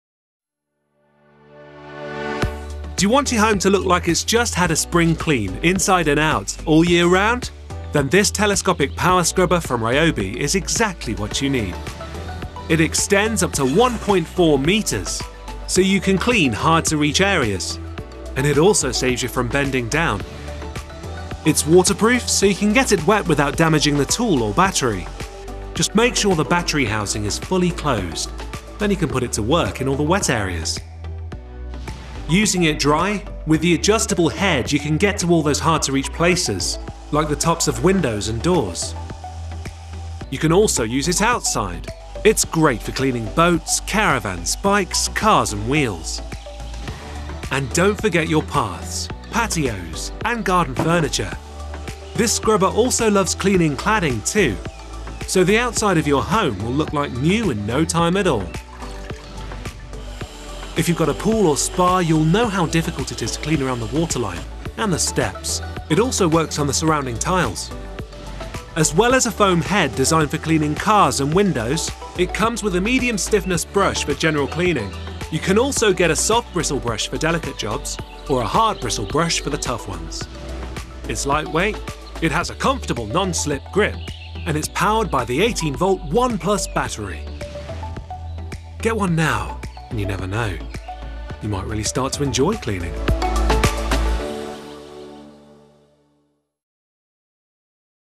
Teleskopisk rengöringsborste 18V ONE+ [R18TPS]
För husägare som vill göra städningen enkel och bekväm även när stora ytor ska rengöras, som t.ex. garageporten, fasaden, båten eller husvagnen. Vår teleskopiska rengöringsborste ger dig 1,4 meter extra räckvidd och ett 15cm stort roterande borsthuvud. Borsthuvudena kan bytas ut till flera varianter som passar det du ska rengöra perfekt. Läs mer om produkten på vår hemsida